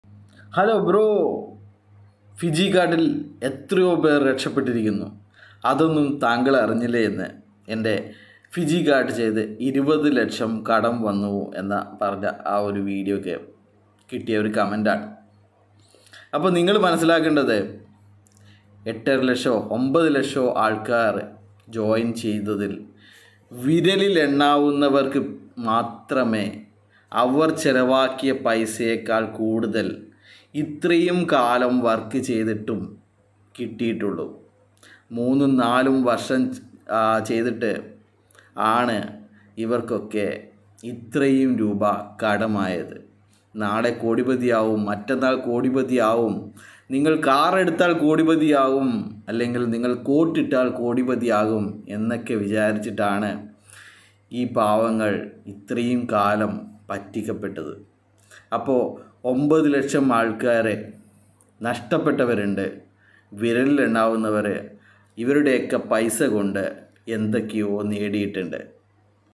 phygicart രക്ഷപെട്ടവരും കടം കയറിയവരും
this video is about phygicart scam